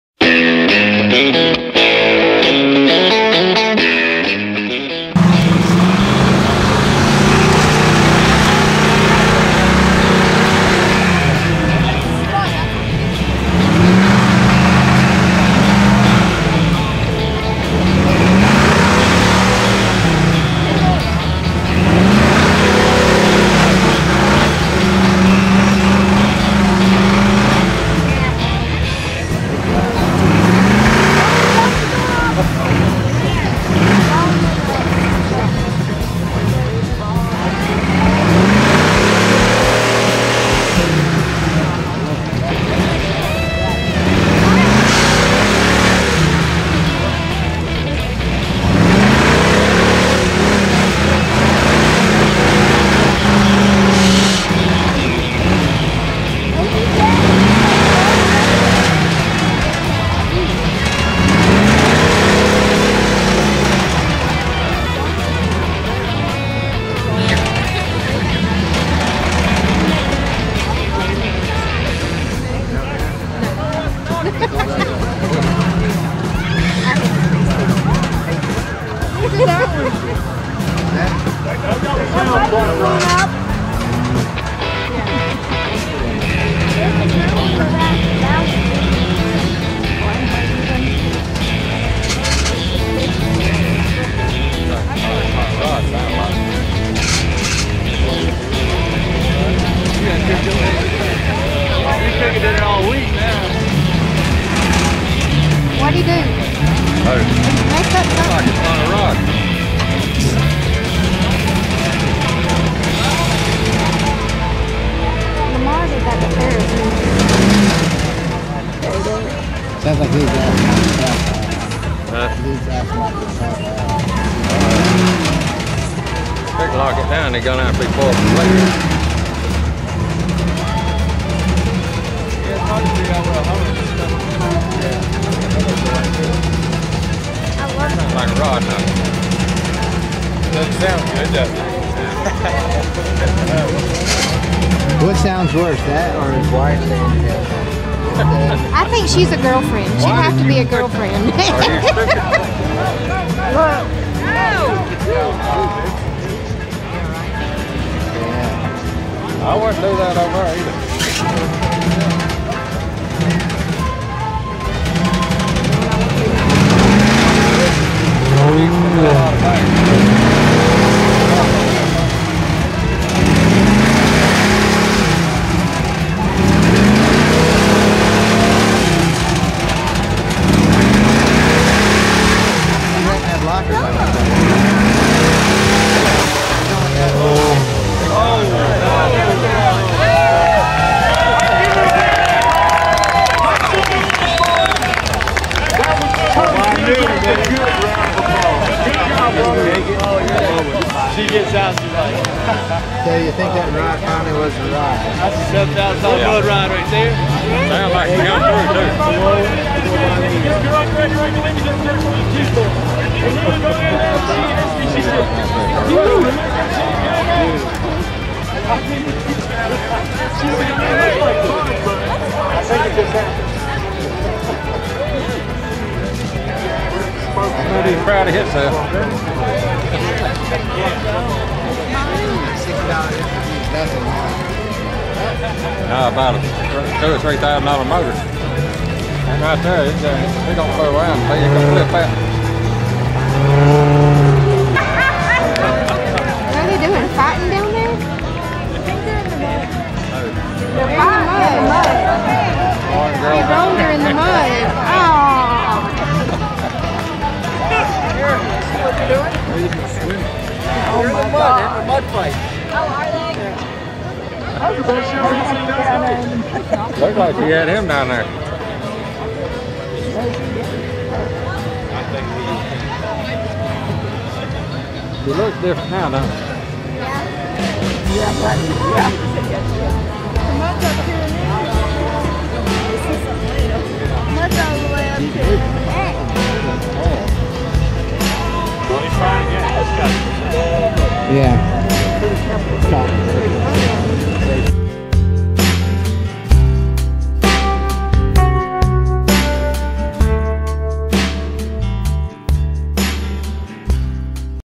JeepFest 2016 Jasper - Jeep engine make a nice smoke cloud -
Jeep engine make a nice smoke cloud follwed by a mud fight!